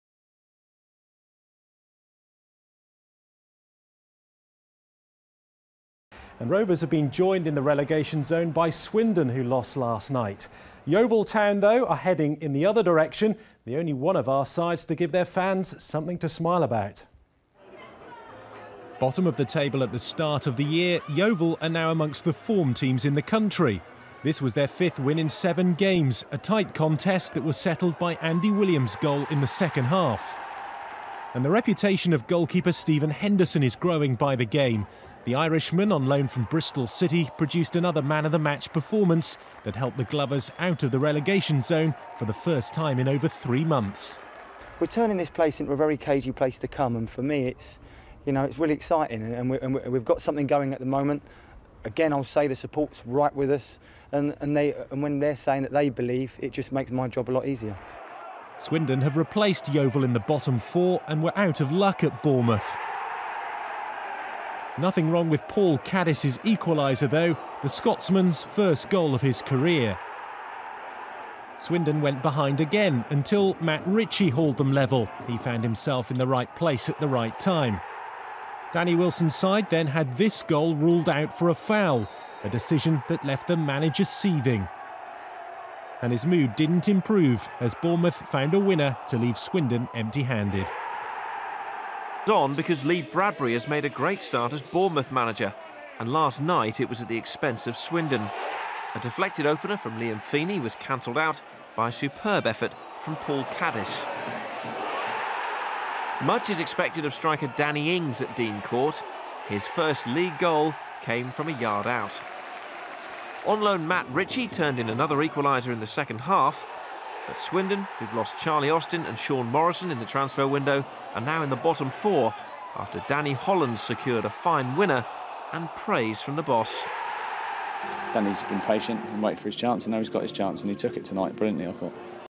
2011-02-01 AFC Bournemouth vs Swindon Town
The Town twice come from behind at Bournemouth, but after Thomas Dossevi has a goal ruled out, the Cherries snatch a winner that sees Swindon drop into the relegation zone.